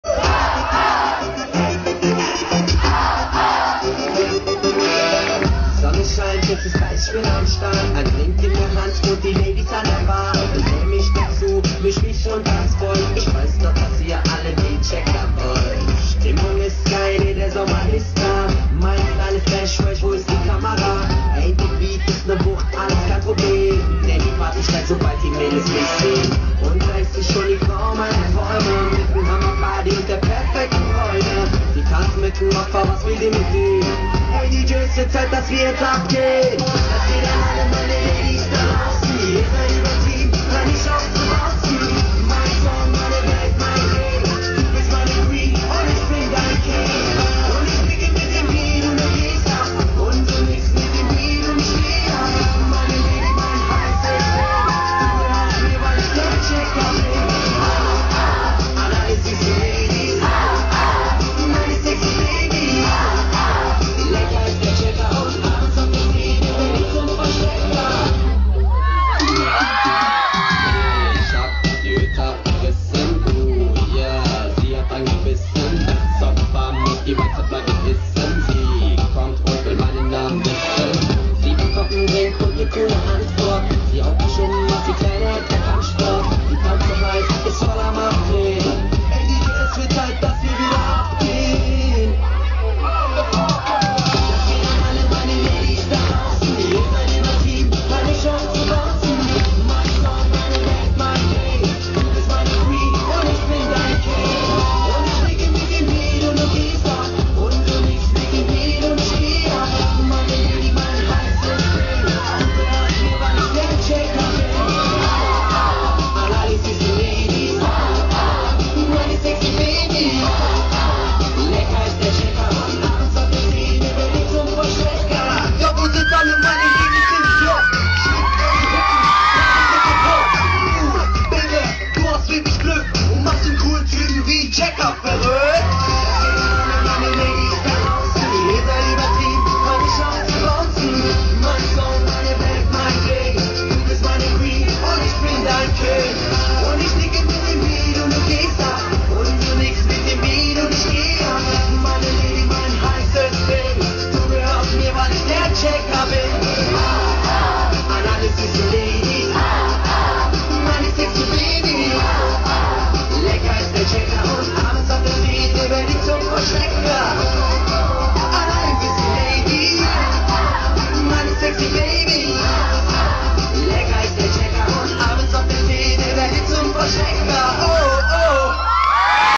Thomas Karaoglan (Toggo Tour 2010) mit Checker, der Vollstrecker
Meine Freundin hat das Meet&Greet mit ihm gewonnen und ich war dabei & dann hat er noch seine Besten Lieder gesungen bei der Toggo Tour! :)

Have fun!